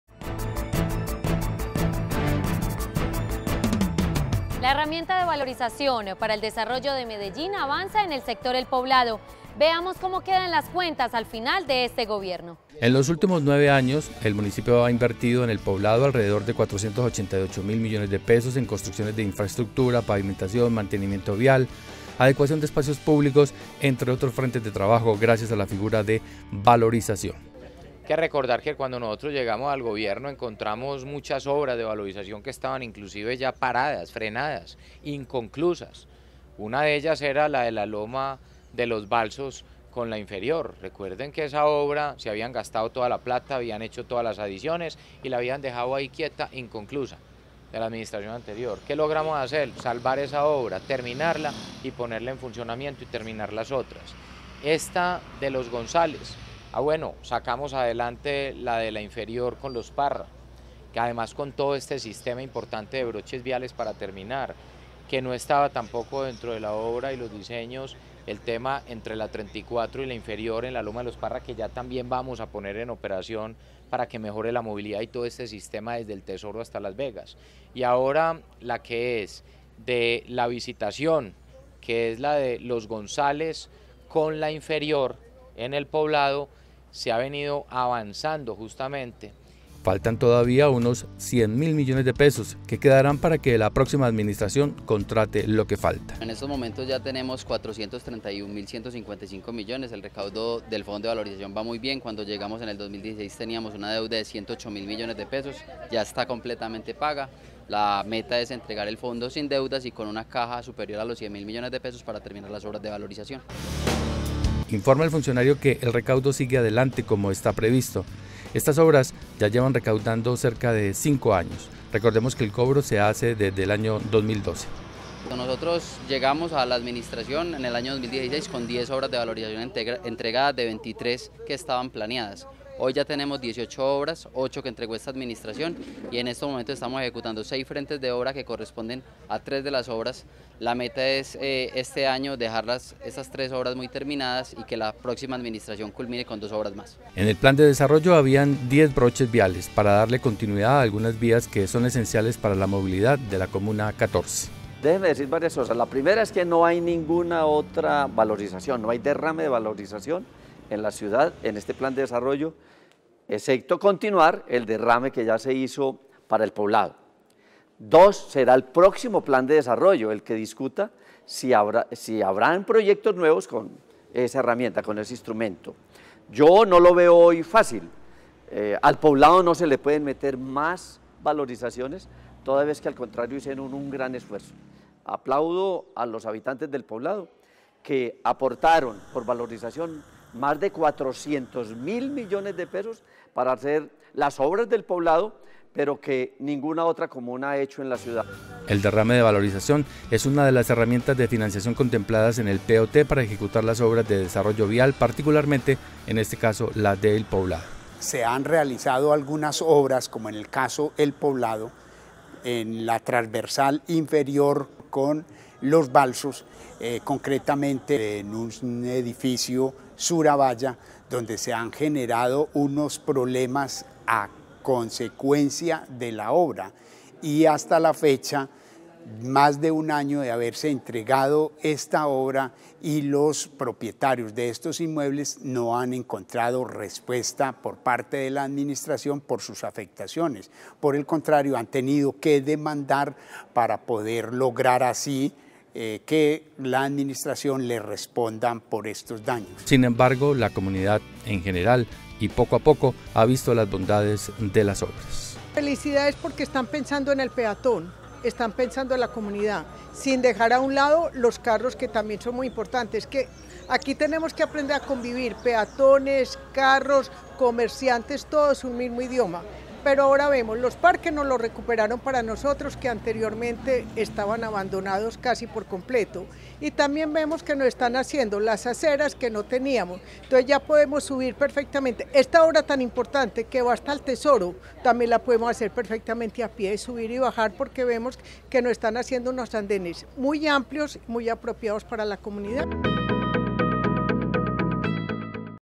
De Acuerdo: entregan obras de valorización en El Poblado
En los últimos 9 años, el Municipio ha invertido en El Poblado alrededor de 488 mil millones de pesos en construcción de infraestructura, entre otros frentes de trabajo gracias a la figura de la valorización.